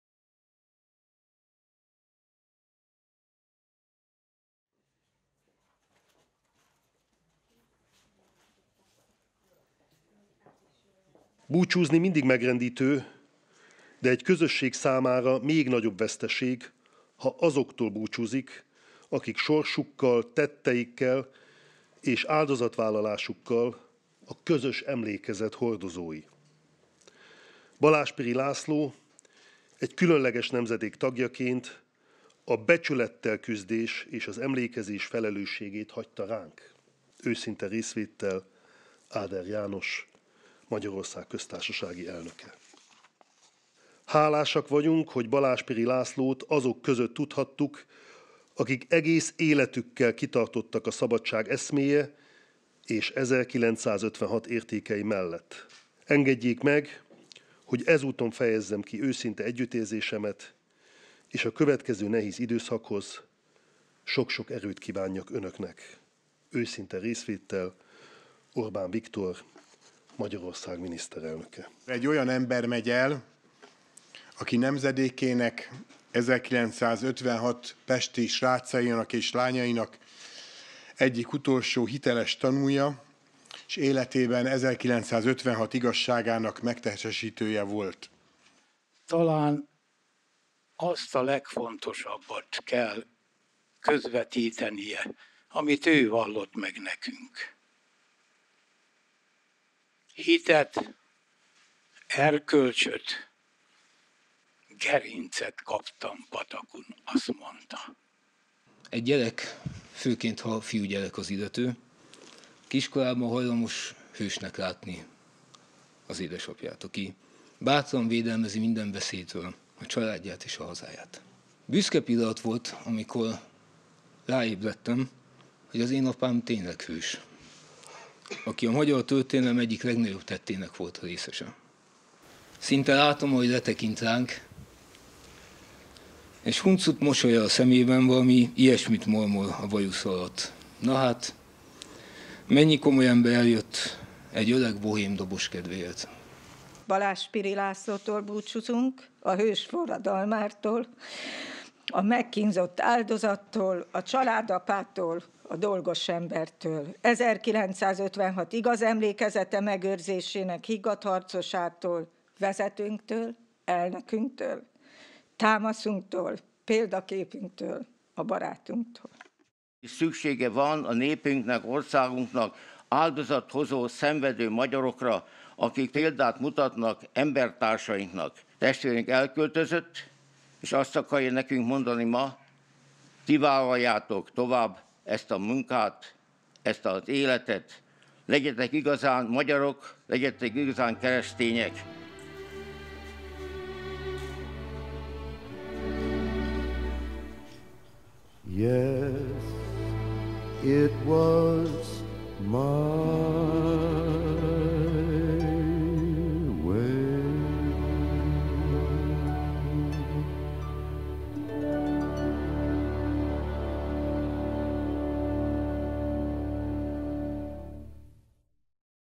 Végső búcsút vettek Balás-Piri László 1956-os szabadságharcostól
A farkasréti temetőben búcsúztatták el hozzátartozói, barátai, valamint munkatársai Balás-Piri László 1956-os szabadságharcost, a Terror Háza Múzeumot is működtető Közép- és Kelet-európai Történelem és Társadalom Kutatásáért Közalapítvány kuratóriumának elnökét, a Történelmi Igazságtétel Bizottság alelnökét, az 1956 Alapítvány kurátorát.